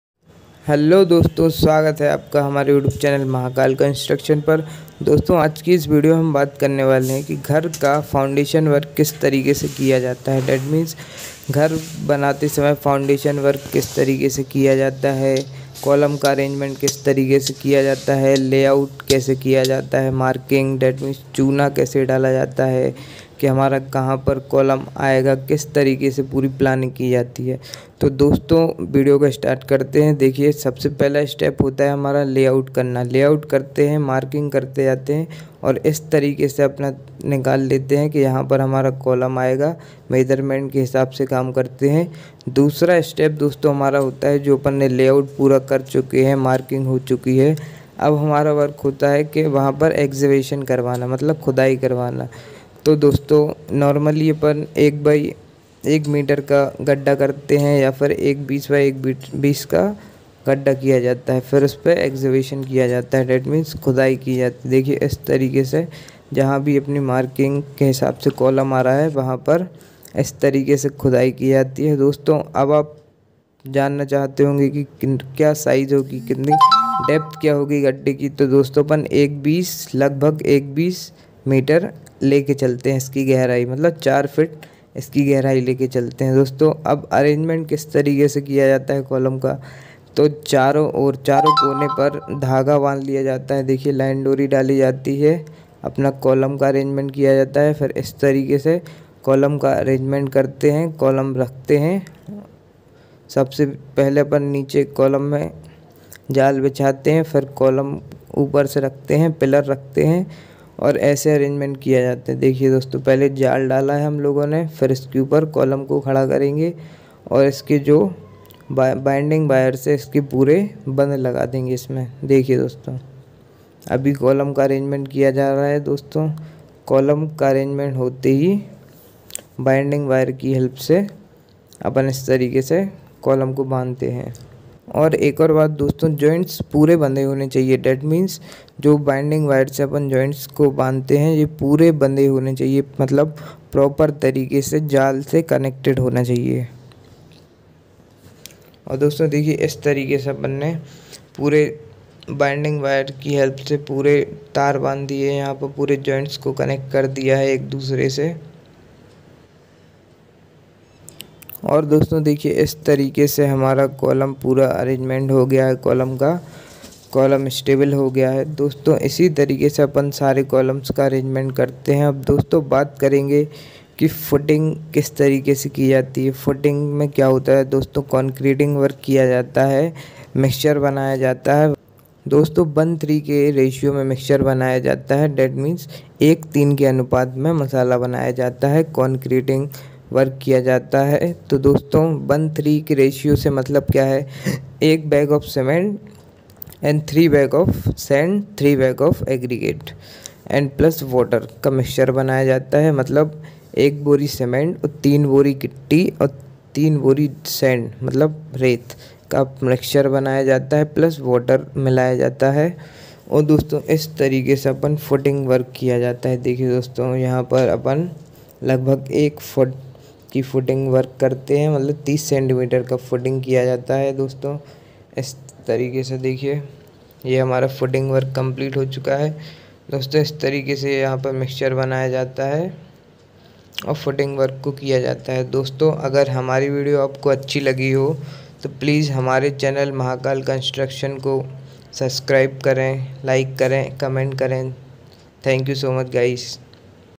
Foundation Work, How to Layout House foundation, Column Layout, Footing Rcc
Topic -  घर के फाउंडेशन का लेआउट कैसे करते है ? How to layout House foundation , Column layout , footing Rcc work